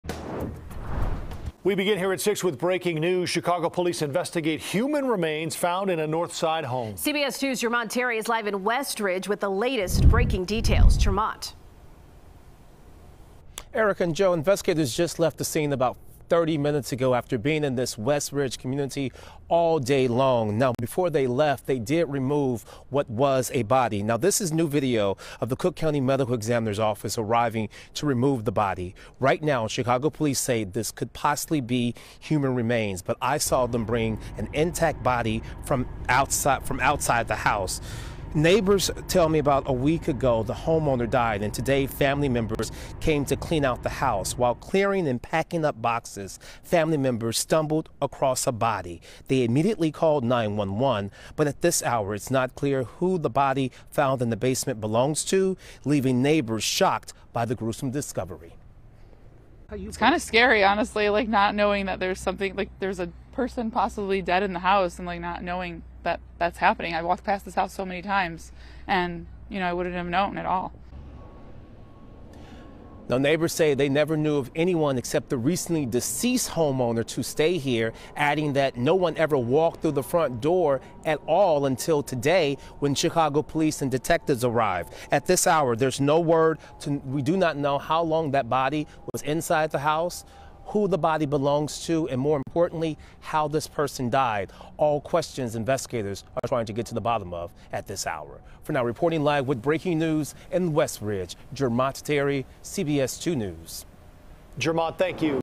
Human remains found in house in Chicago's West Ridge community
Before the scene was cleared, investigators from the Cook County Medical Examiner’s office came to the scene to remove an intact body from the house. CBS 2's Jermont Terry reports.